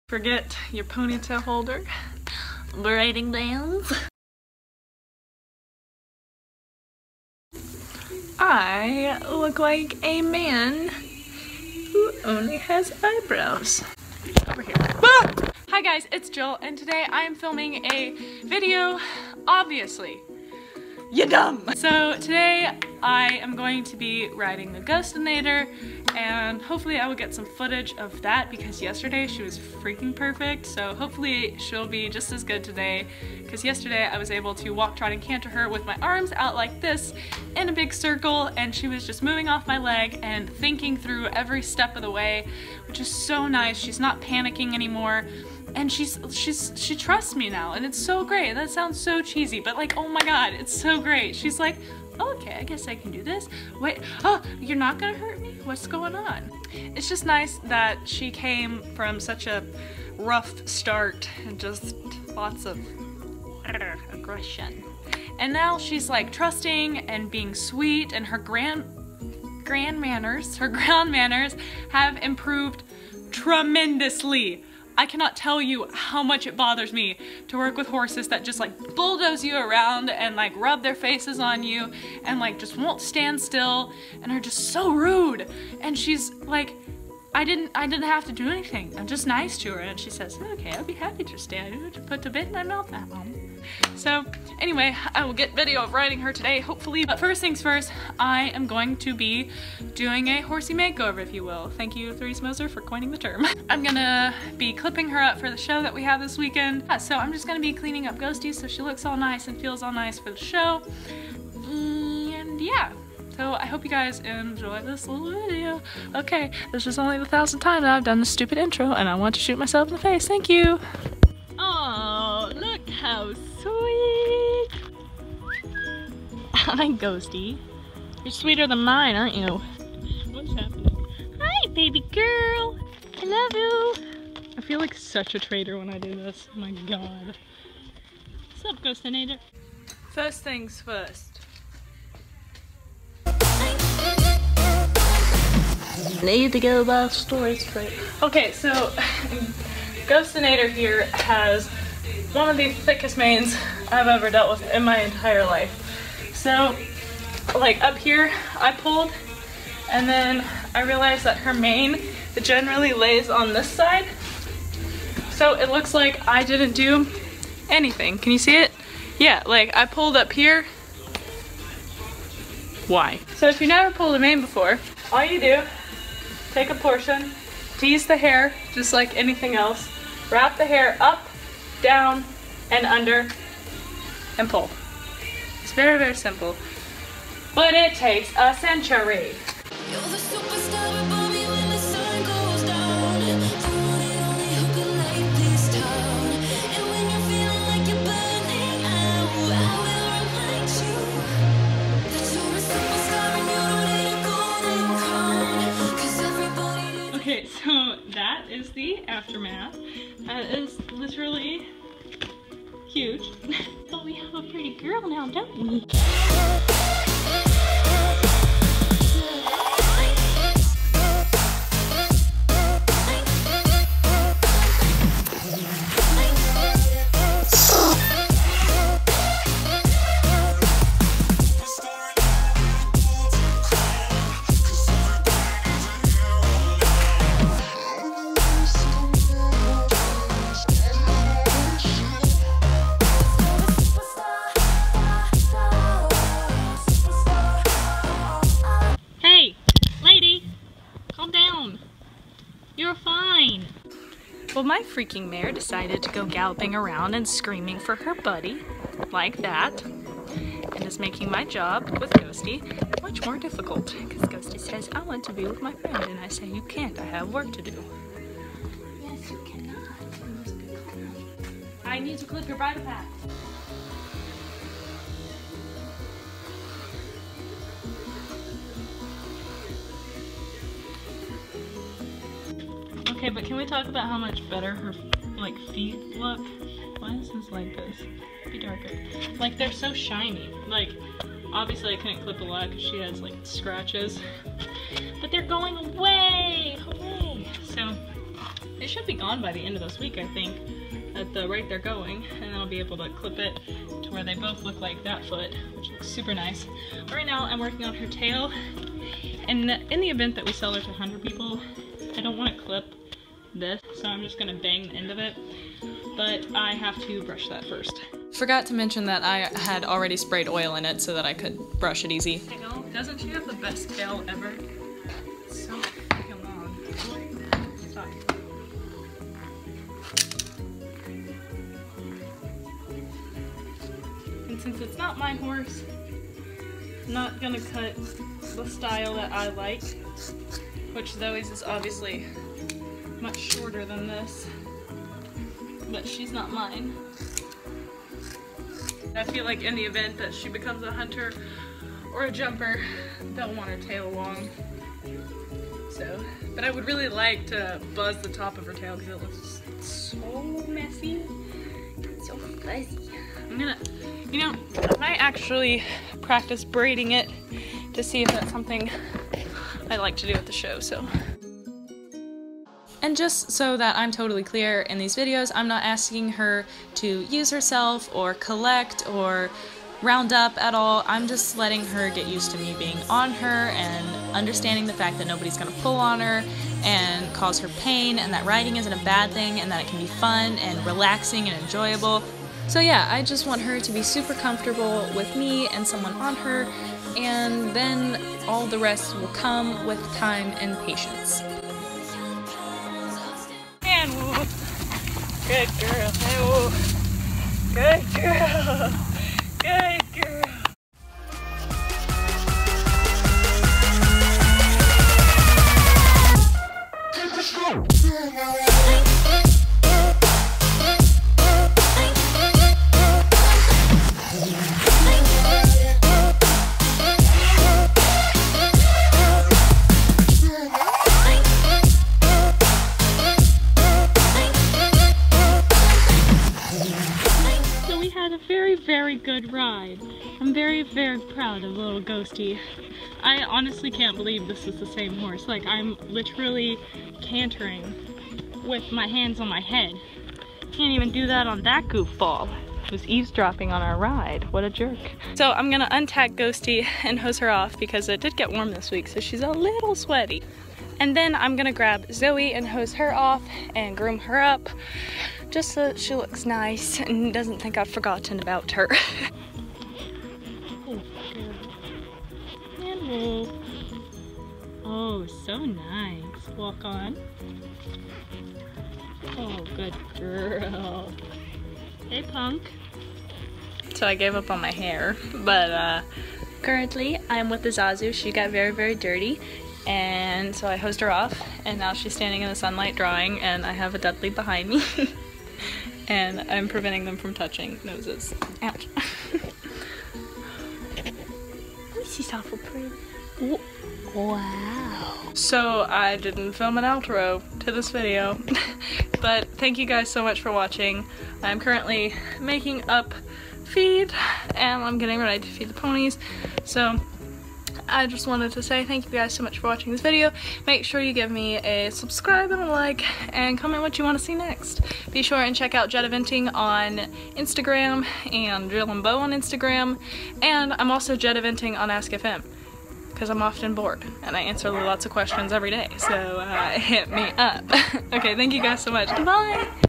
GHOSTY GLO UP & RAWS // BARN VLOG #5
ahh i couldnt be moreee proud of this little mare! ive been enjoying filming so much lately ahhh i cant wait to film the show vlog and put some jump videos together for you guys and hopefully rehome this incredible mare!